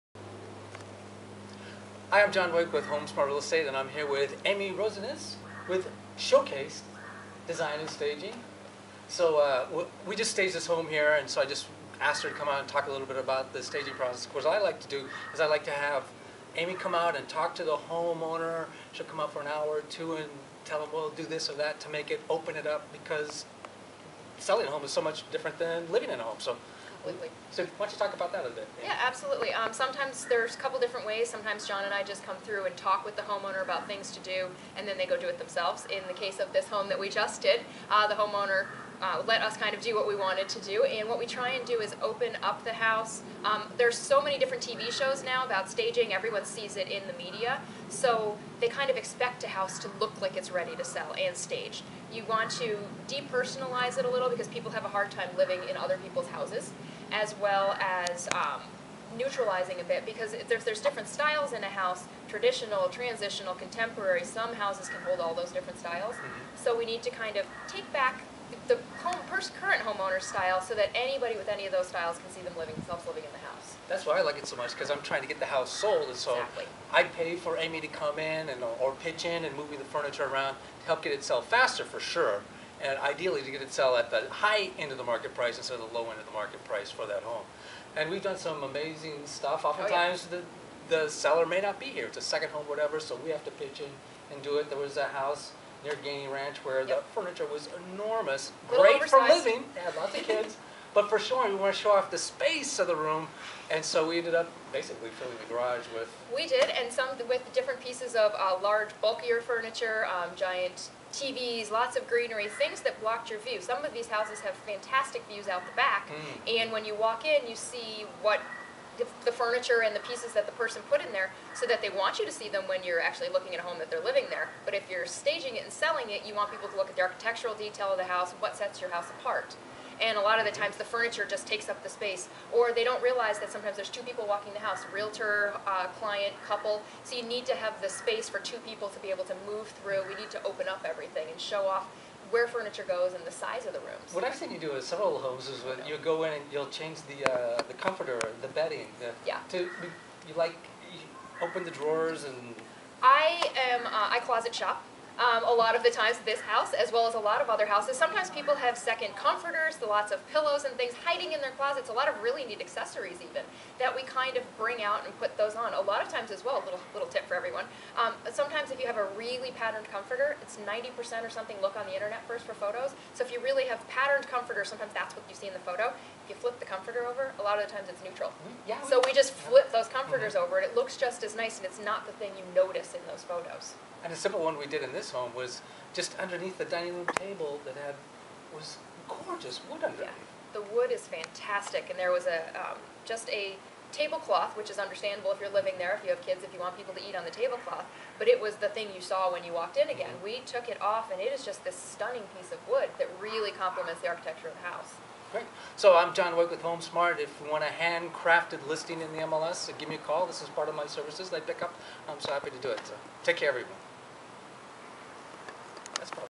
Scottsdale Home Staging Tips
Originally uploaded Mar 24, 2012.
Home staging tips in Scottsdale from Amy Rozines of Showcase Design and Home Staging with John Wake, real estate agent at HomeSmart.

John Wake
Realtor, Associate Broker, M.S.